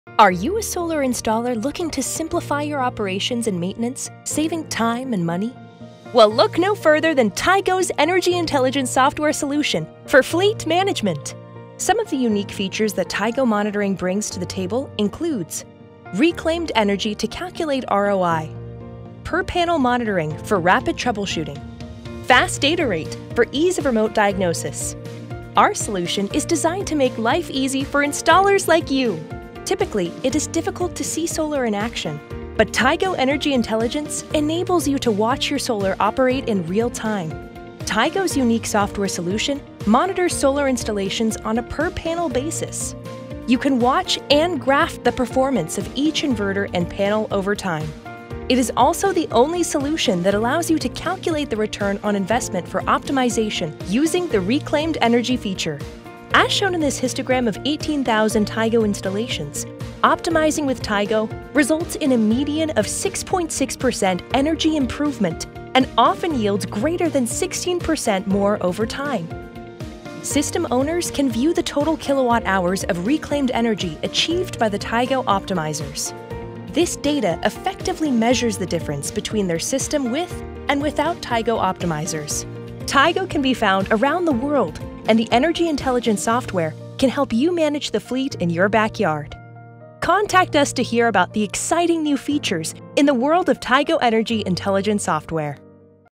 Simplify solar fleet management with Tigo Energy Intelligence
Tigo Energy Intelligence gives you detailed visibility into module-level operations so you can assess performance characteristics, diagnose issues. Reduce truck rolls and get peace of mind that your system is performing the way you designed and installed it.